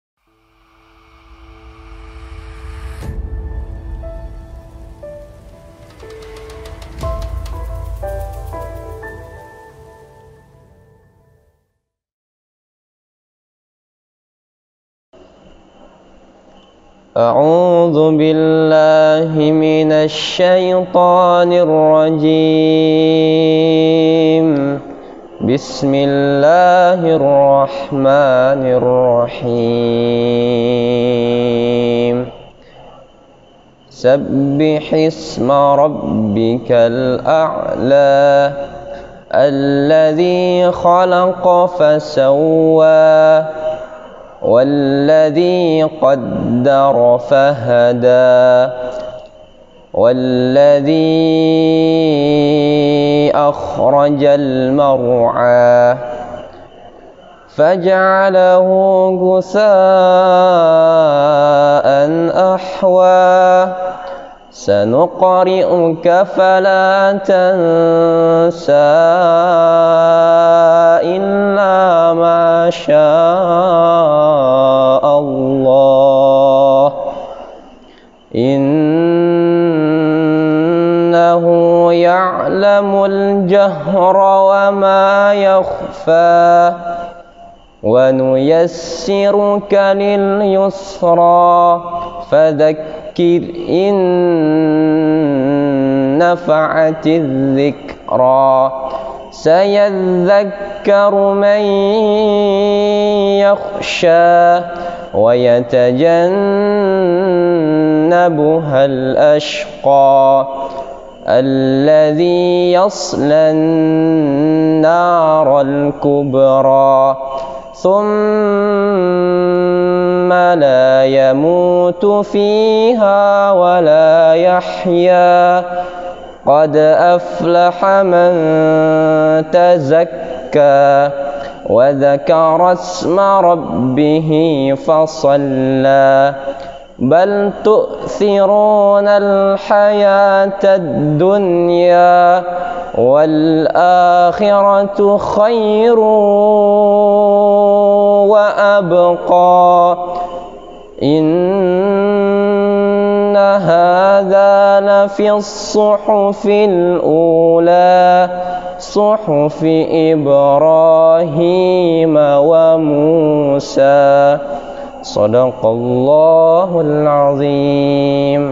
Ust. M. Royyan Ramdhani - SURAH AL-A'LA (Yang Paling Tinggi) - Surah ke 87 Makkiyah
Ustadz M. Royyan Ramdhani dari Cepak Lauq Aikmel,  Salah satu Pendidik & Pengajar di DIBAMA Aikmel.